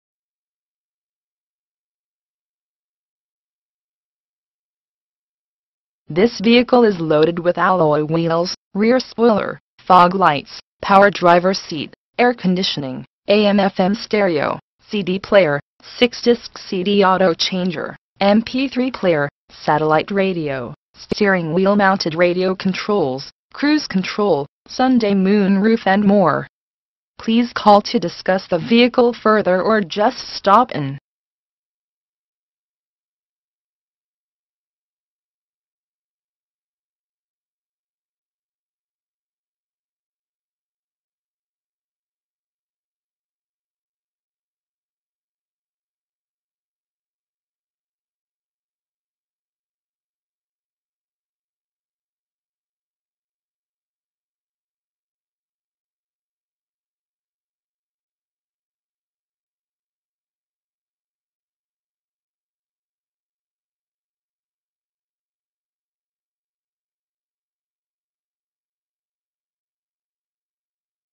2009 Dodge Avenger - Barnett Chrysler Jeep Kia Used
For Sale: 2009 Dodge Avenger
VIN: 1B3LC56D69N522922
Engine: Gas V6 2.7L/167
Drivetrain: FWD
Transmission: Automatic
Mileage: 32,416
Color: Deep Water Blue Pearl (exterior) Dark Slate Gray (interior)

Features:
This vehicle is loaded with ALLOY WHEELS, REAR SPOILER, FOG LIGHTS, POWER DRIVER SEAT, AIR CONDITIONING, A M/FM STEREO, CD PLAYER, 6-DISC CD AUTO CHANGER, MP3 PLAYER, SATELLITE RADIO, STEERING WHEEL MOUNTED RADIO CONTROLS, CRUISE CONTROL, SUN/MOON ROOF and more! Please call to discuss the vehicle further or just stop in!